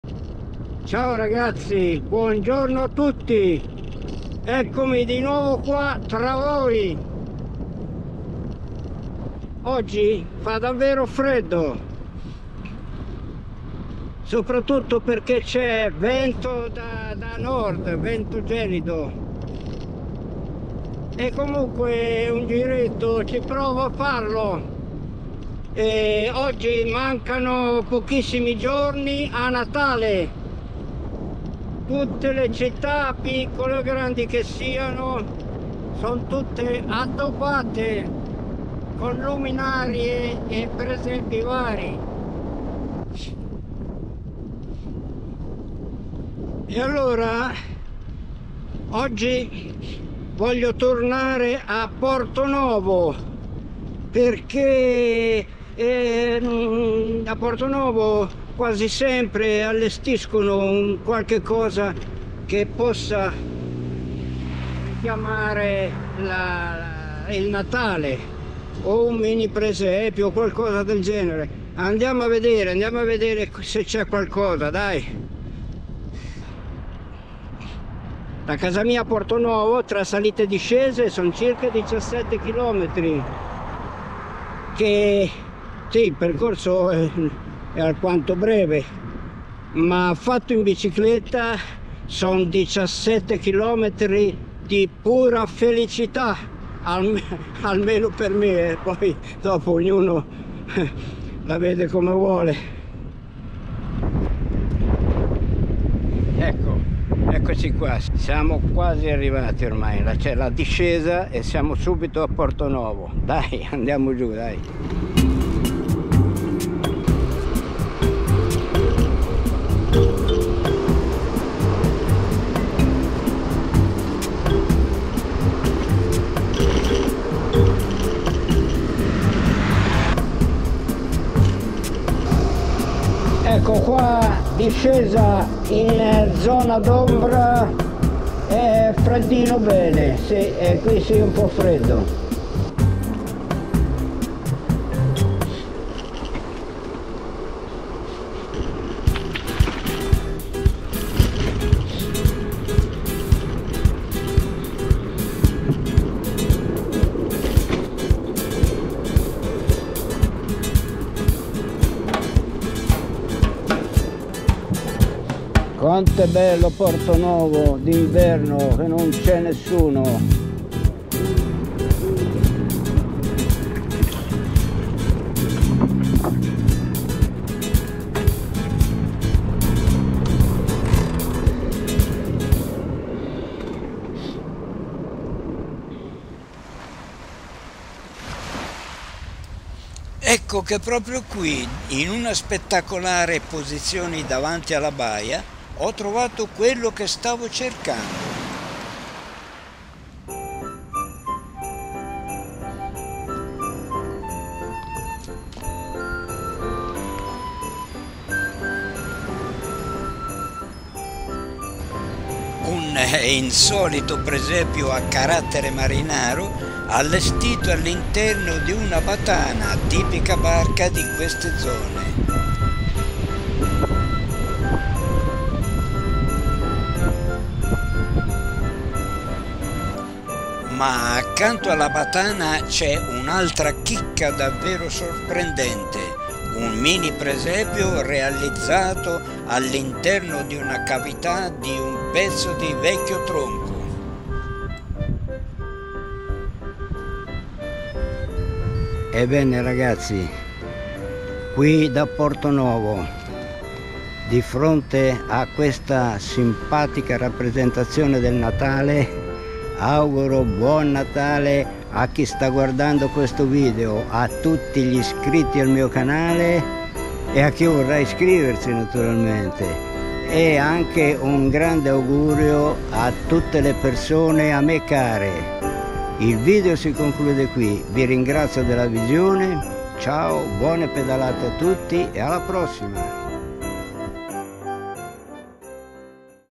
Buon Natale da Portonovo🎄🚴​🚴‍♀️​
Mancano pochissimi giorni a Natale e vado a Portonovo per scoprire se hanno allestito qualcosa che richiami il Natale.
Ho trovato un fantastico presepio dentro una battana e un mini presepio dentro la cavità di pezzo di vecchio tronco d'albero.
Insomma il luogo ideale per porgere gli Auguri di Buin Natale a tutti voi!🎄